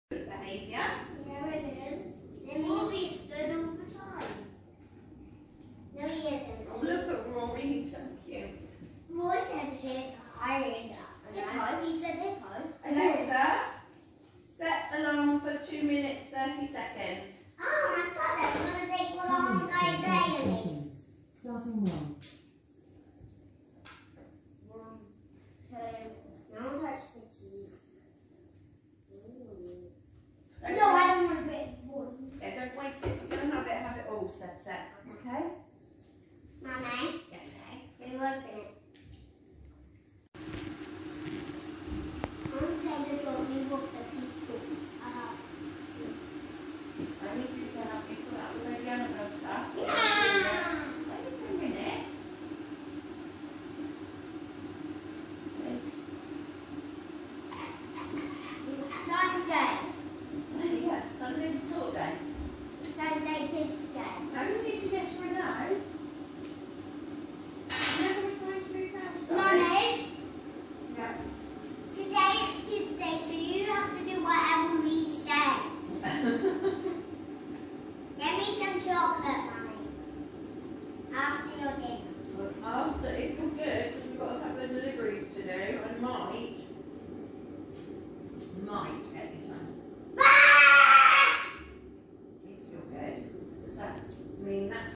👍 Thumb twitching. Nerve issue? 👍
Thought I'd upload a short video of my thumb twitching. Happens every few days, and can last a few seconds to a few minutes.